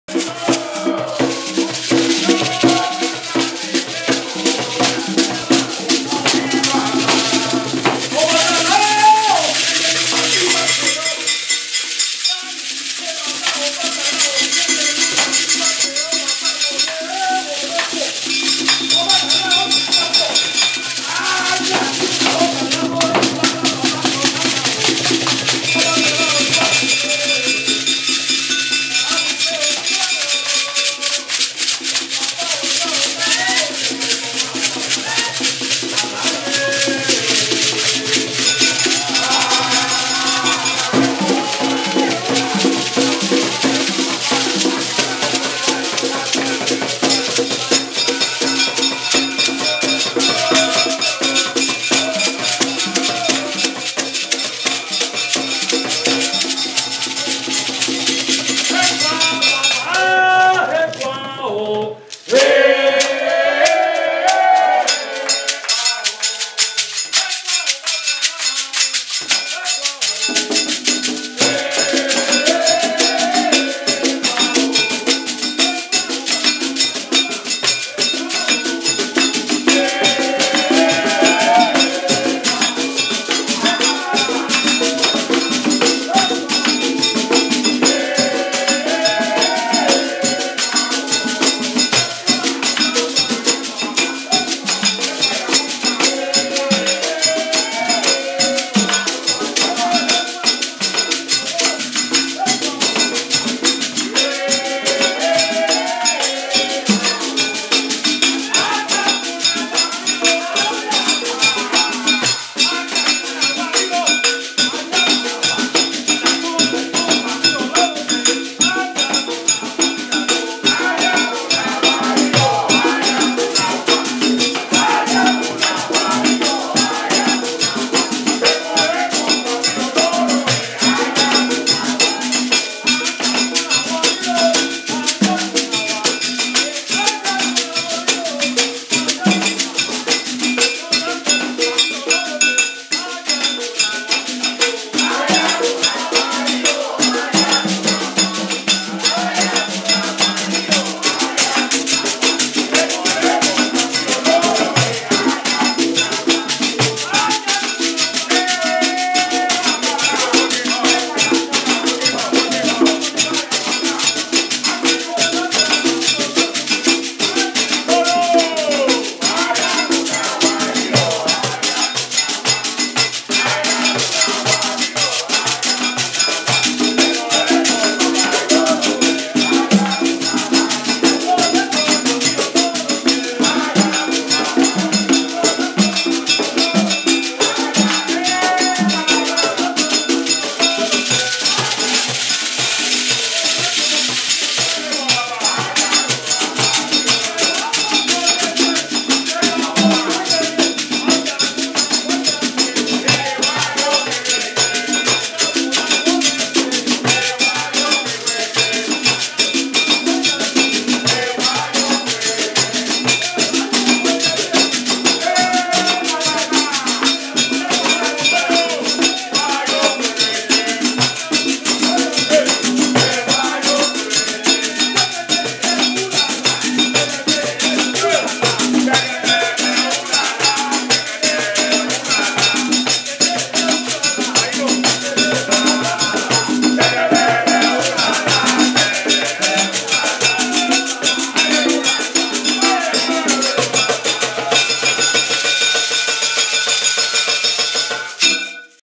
Guiro para Oshun en casa de Iris 4/25/15 Obatala taken by Reese
Starring Okanilu Group